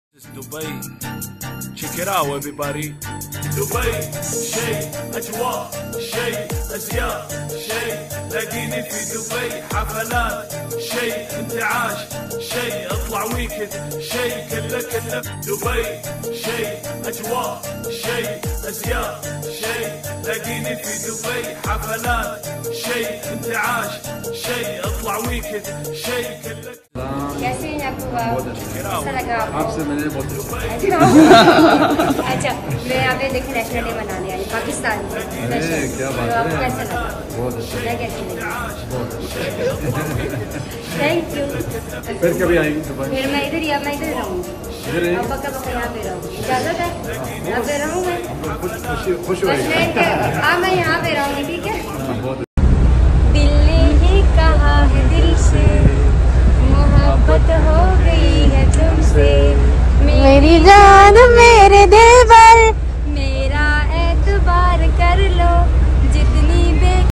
Sandal Khattack in Dubai with Sheikh
Sandal Khattack
‏ہم تو اپنے دفتر خارجہ کے آفس کو رو رہے تھے اور ان کی پہنچ دیکھیں۔